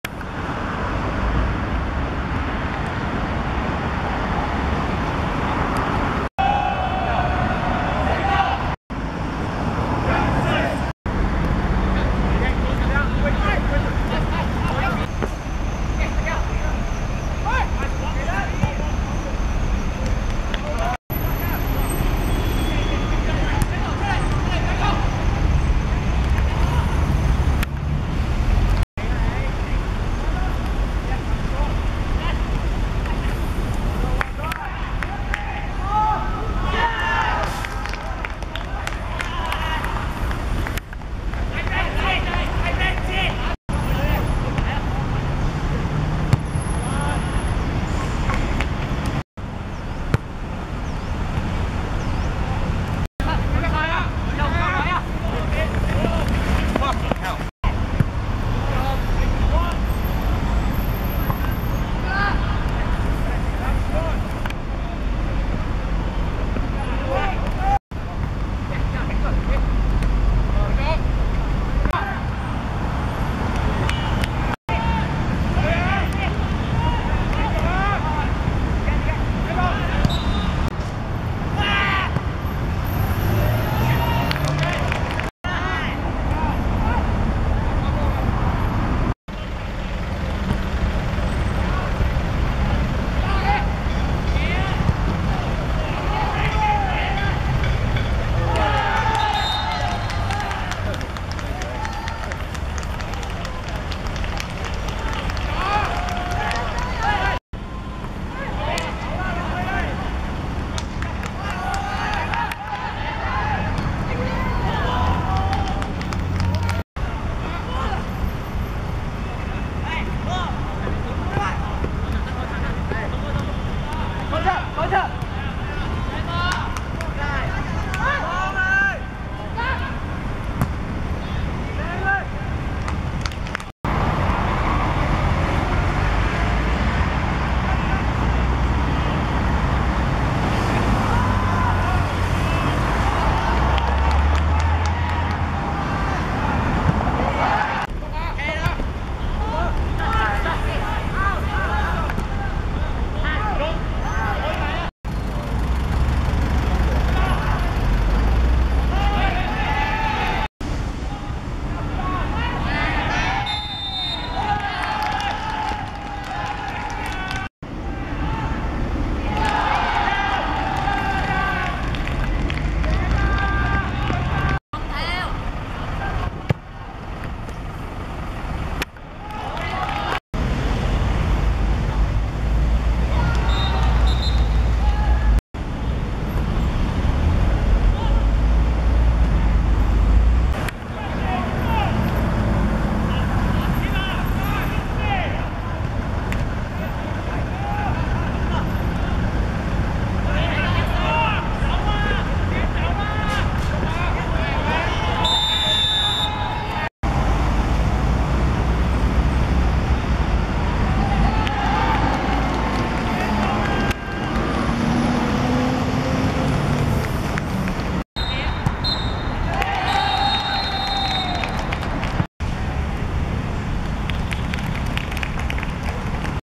聖若瑟英中vs德瑞(2015.2.11.D2學界足球乙組八強)精華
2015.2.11.界限街仿真草塲 17:00 聖若瑟英中(紅白) 3:1 德瑞(綠)